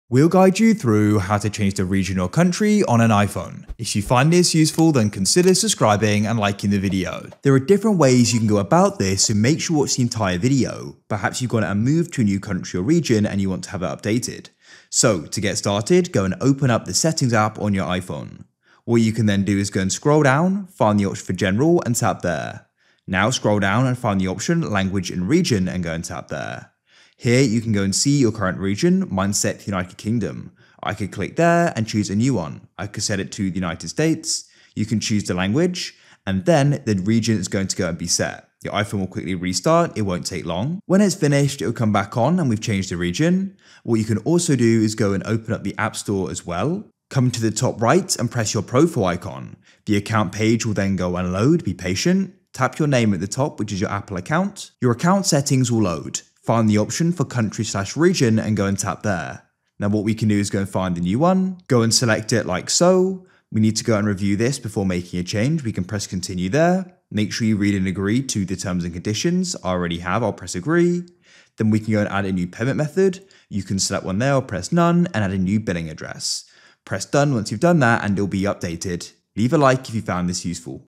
How To Change Region In iPhone - Full Guide
Learn how to change region or country in iphone in this video.

GuideRealm